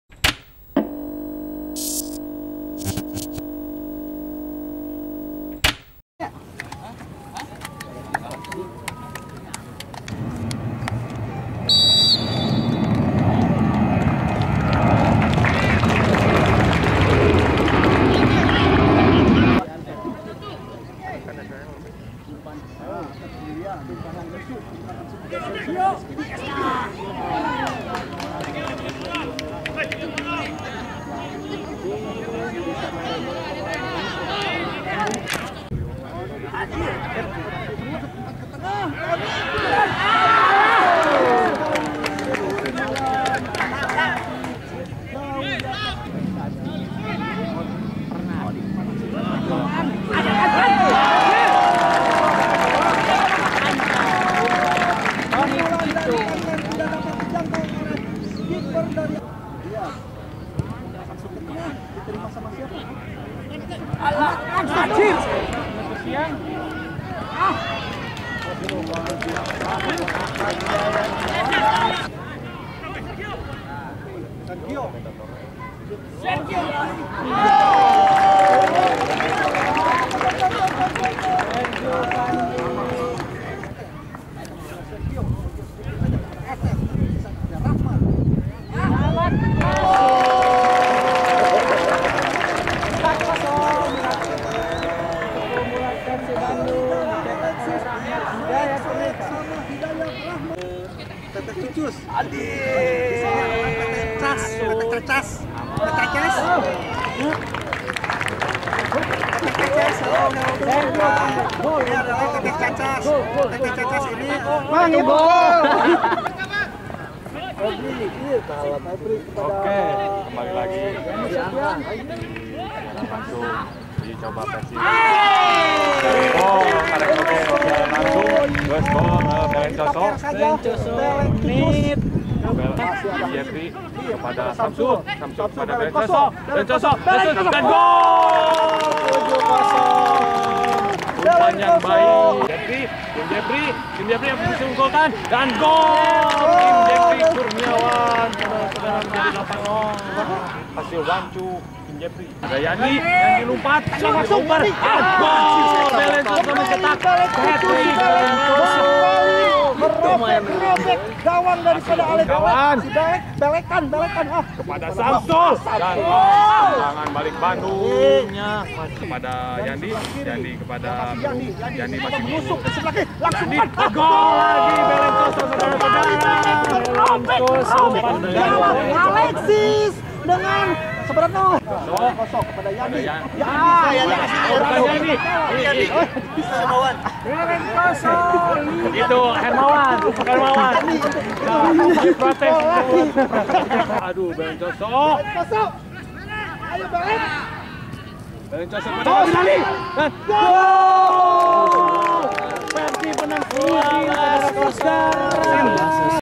UJICOBA PERSIB - Highlight Laga Uji Coba Persib vs Alexis FC
Persib Bandung mengisi lowongnya jadwal pertandigan Torabika Soccer Championship 2016 dengan partai uji coba. Rabu (1/6), Persib menggelar pertandign uji coba melawan Alexis FC. Persib memenangi pertandingan dengan memberondong selusin gol, 12-0.